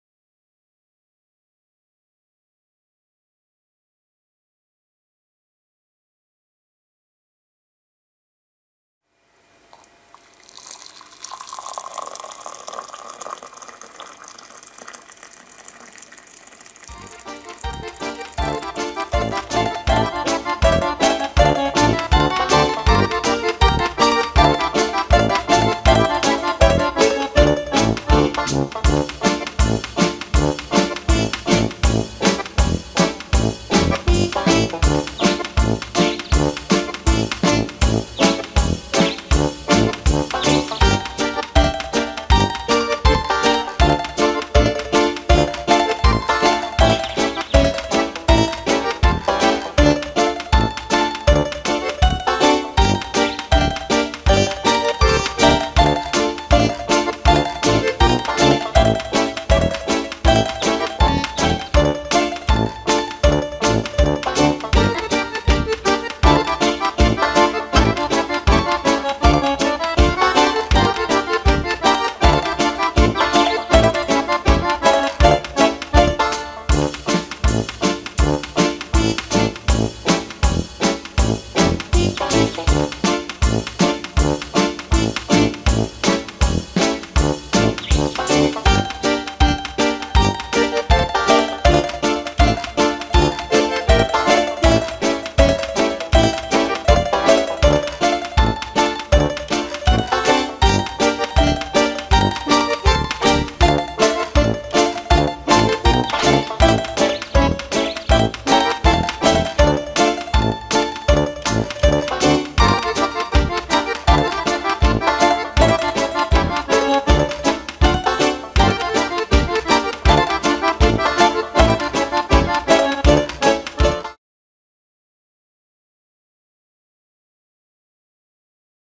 magiczny kubek
magiczny kubek, kubek zmieniający się podczas wlewania wrzątku